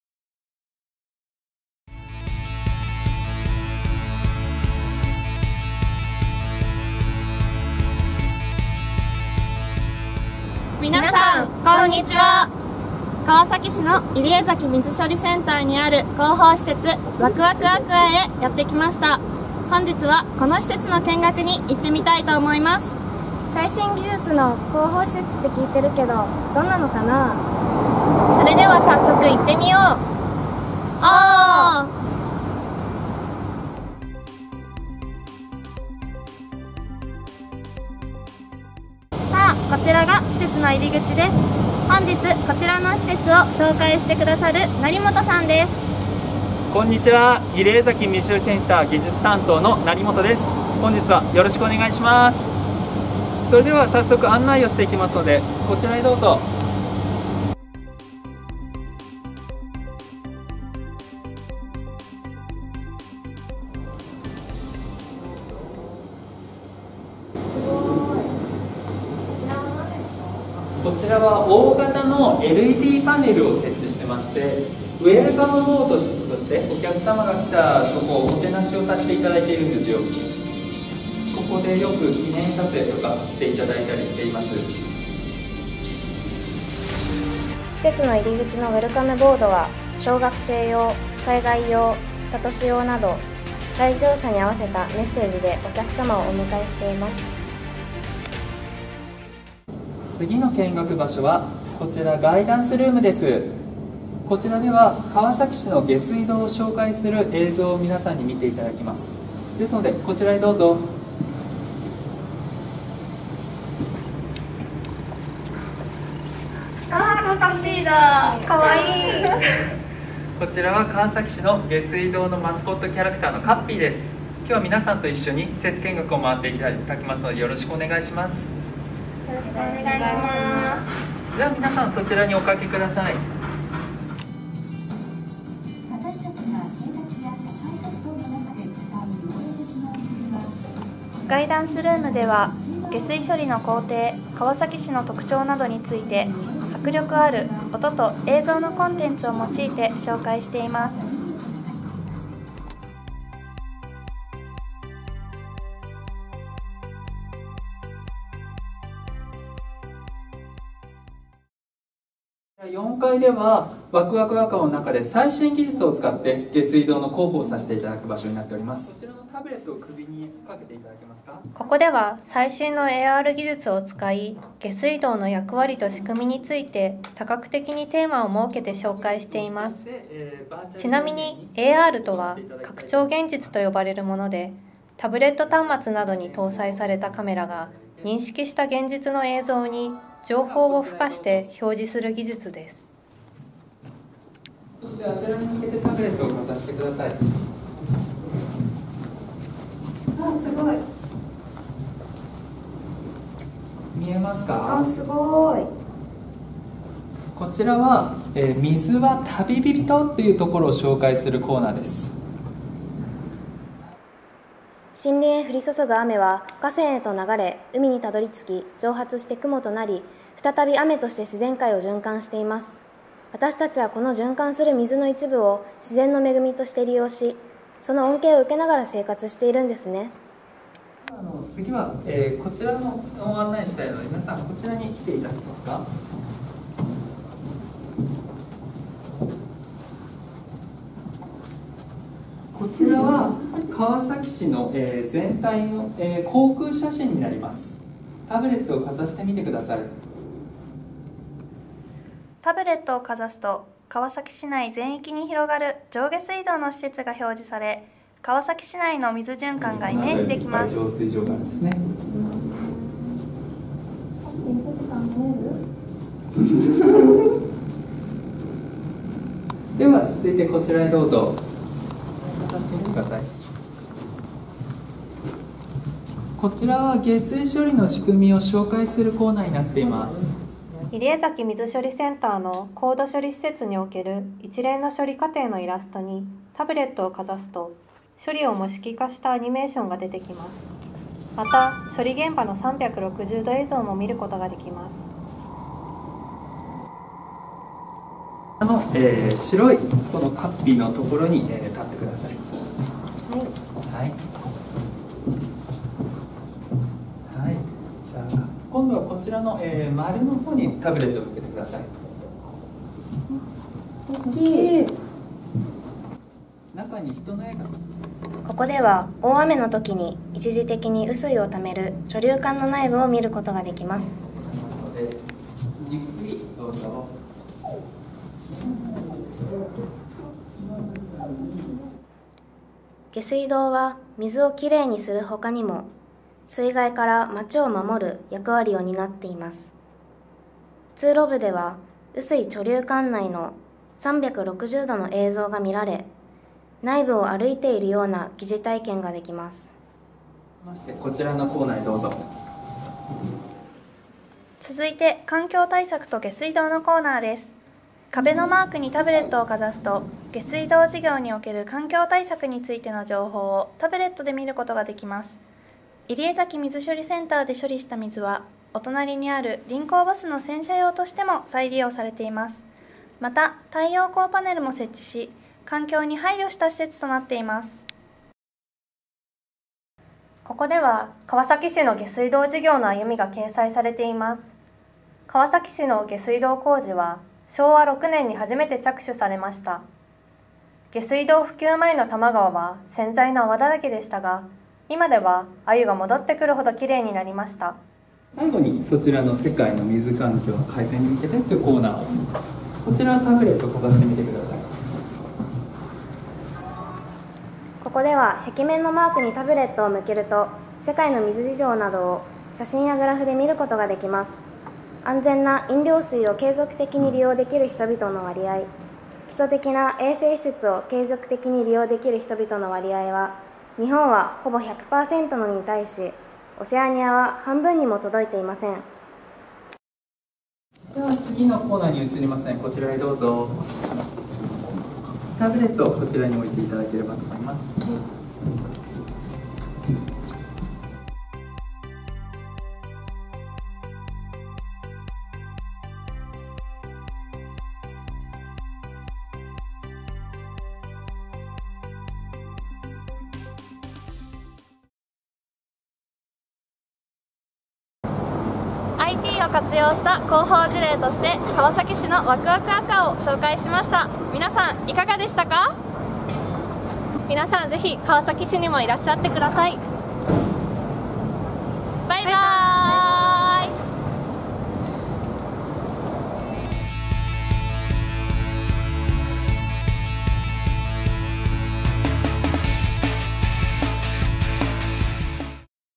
かわさき下水道広報施設ワクワクアクアへ行ってみよう♪
入江崎水処理センター（川崎区）にある下水道広報施設ワクワクアクアの施設紹介動画です。
2019年4月にオープンしたワクワクアクアは、水循環の中での下水道の役割と大切さについてバーチャル体験できる広報施設となっています。市職員がリポーター役となり実際に体験している様子をぜひご覧ください。

～この動画は、「下水道分野で働く女性の会（ＧＪリンク）」で開催された令和２年度オンライン全国ワークショップでの視聴用コンテンツとして、ＧＪリンクから制作依頼されたものを再編集しています。～

関連するホームページ
かわさき下水道広報施設 ワクワクアクア（入江崎水処理センター広報施設）の施設見学について
URL：

川崎市上下水道局下水道部入江崎水処理センター